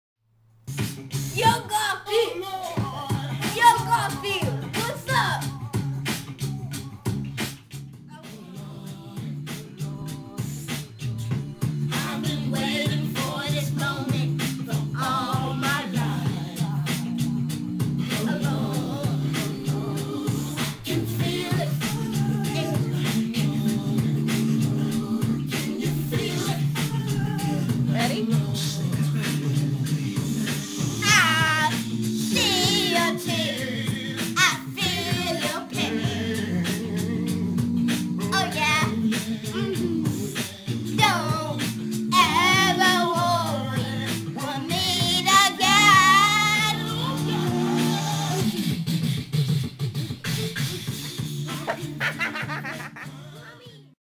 Kaleb's Tribute to N7
this is my son Kaleb..  i've played N7's CD sooo much that my son has inadvertantly learned the words to the song.. of course i didn't realize this until i was on my computer and he came up behind me singing the song.  after watching it, i decided it was too funny not to post!!  so, in the spirit of Dwight's replacement.. enjoy!!

BTW..  he was 5 yrs. old at the time.